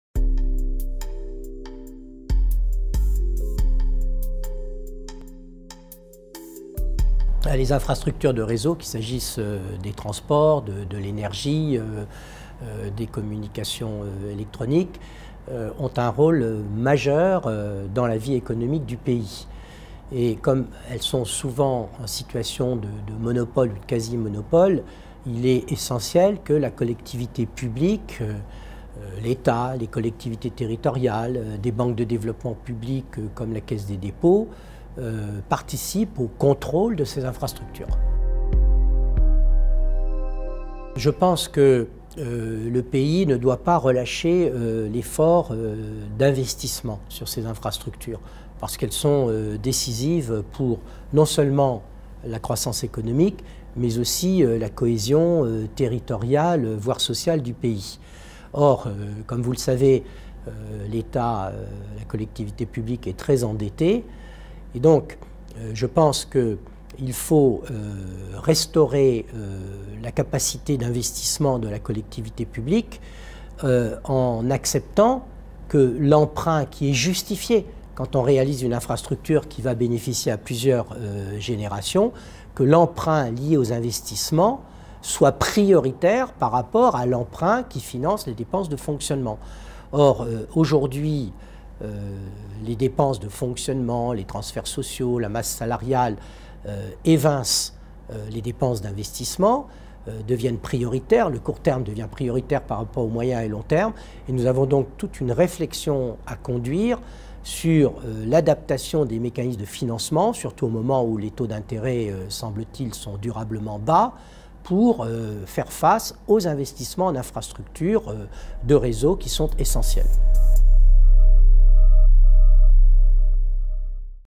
Questions à Gilles CARREZ (Député) - infrastructures de réseaux - cese
Questions à M. Gilles CARREZ, Député du Val-de-Marne (5e circonscription), auditionné par la section des activités économiques du CESE dans le cadre de la saisine : "L'impact des infrastructures de réseaux dans l'économie".